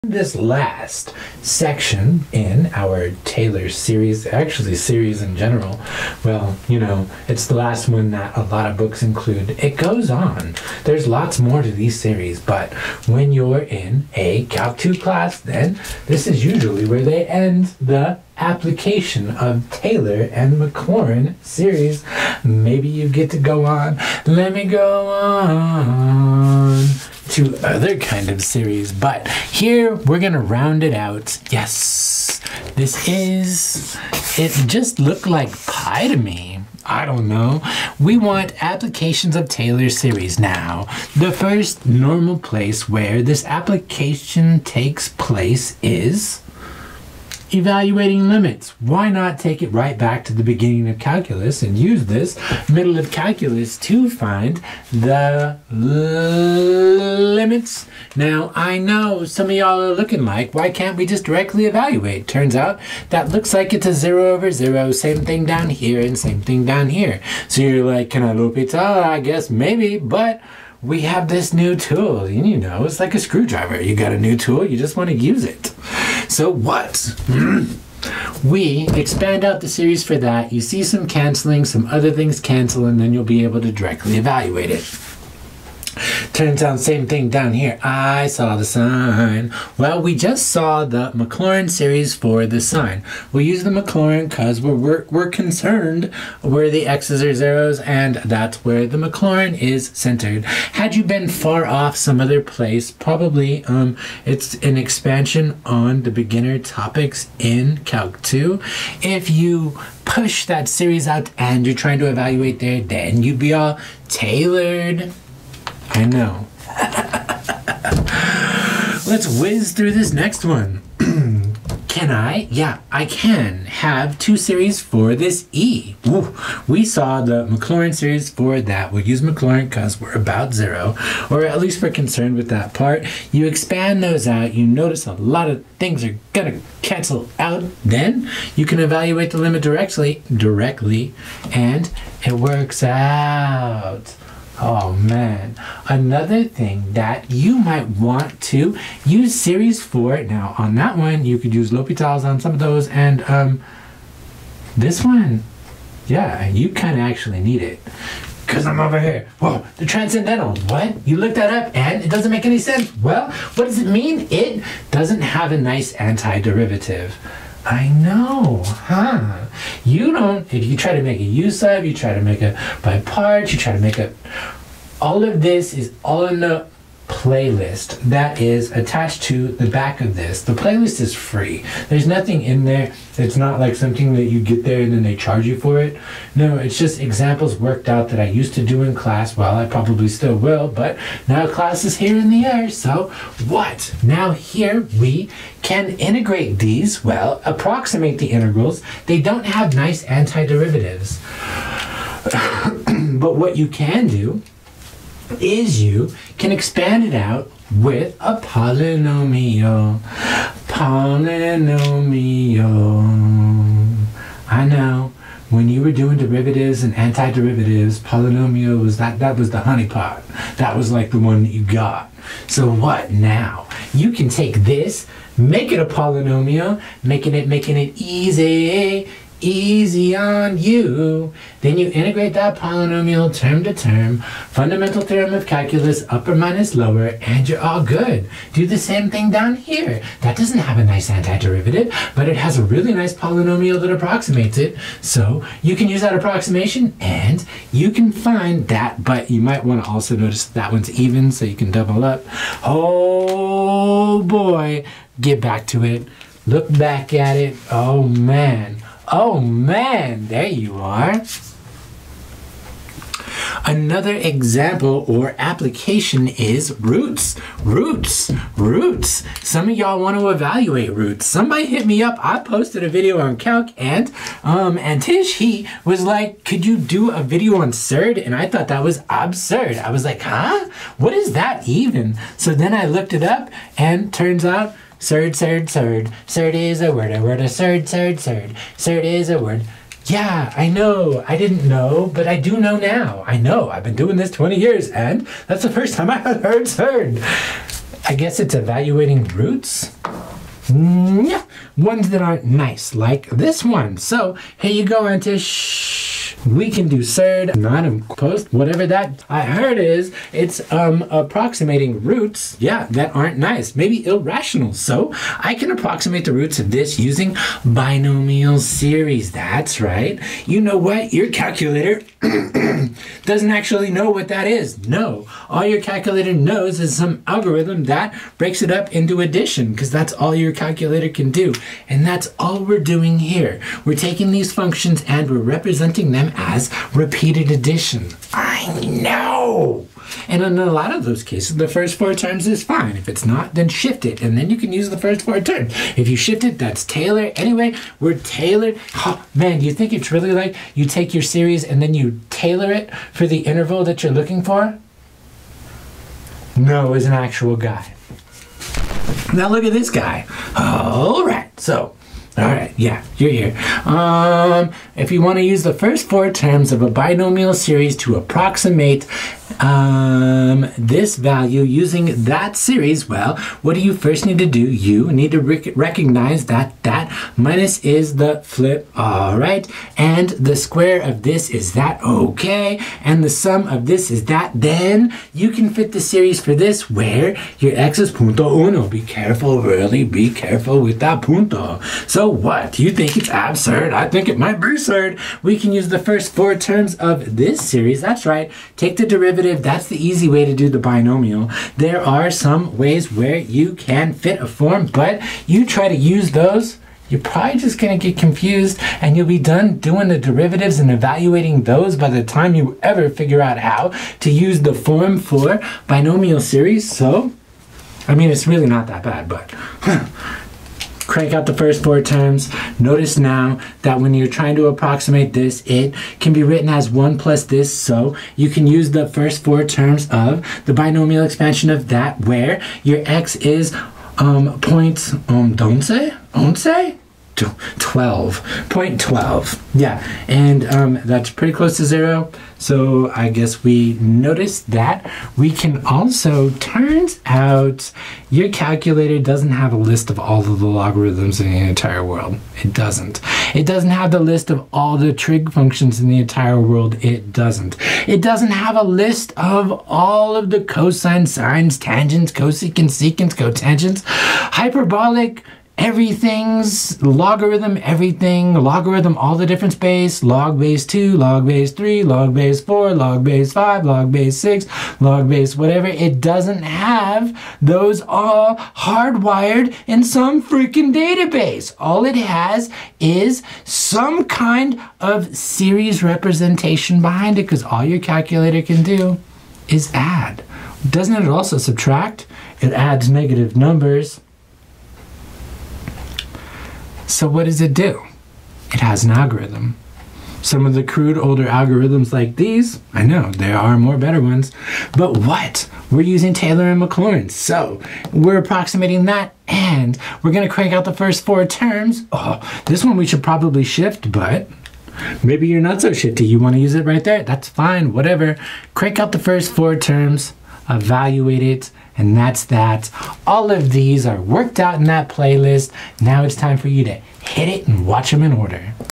The Applications of Taylor and Maclaurin Series Video
This video walks you through the usual applications of Taylor and Maclaurin series. It starts with evaluating limits. I move on to approximating definite integrals of transcendental. Then I talk about approximating irrational roots, Surd. I then round out the talk with approximating function values with series. The video then leads to a playlist of at least 8 examples. No problems are actually worked in this video the problems are linked to this video.